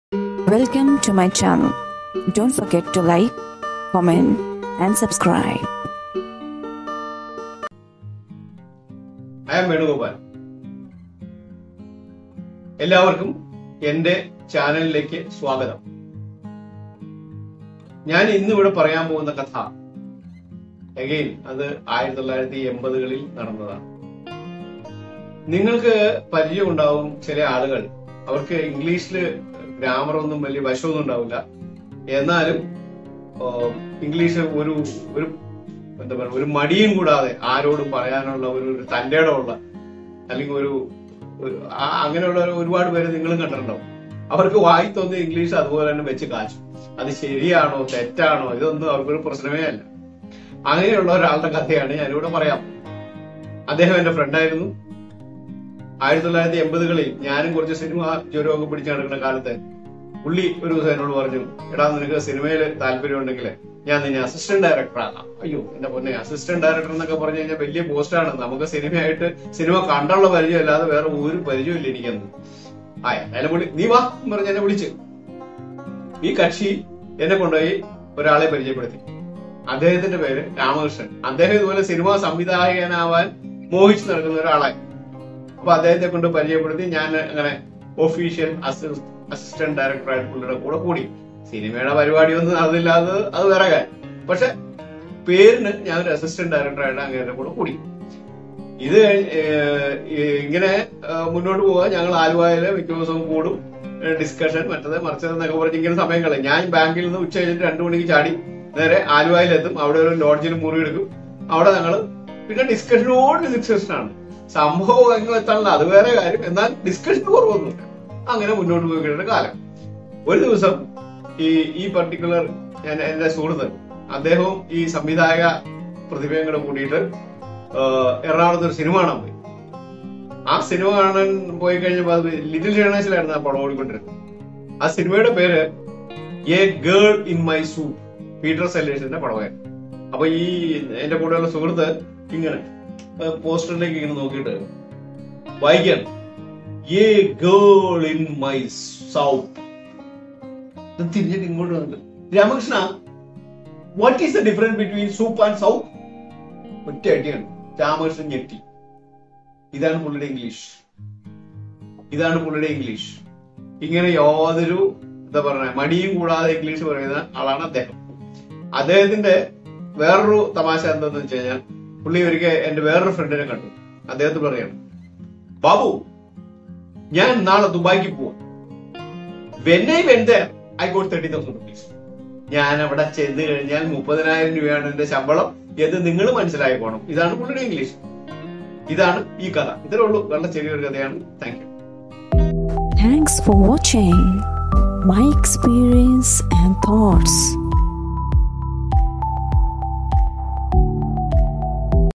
A Girl in my Soup | Peter Sellers | Ramakrishnaa.. What is the difference between soop and soup..
A funny incident happened years back.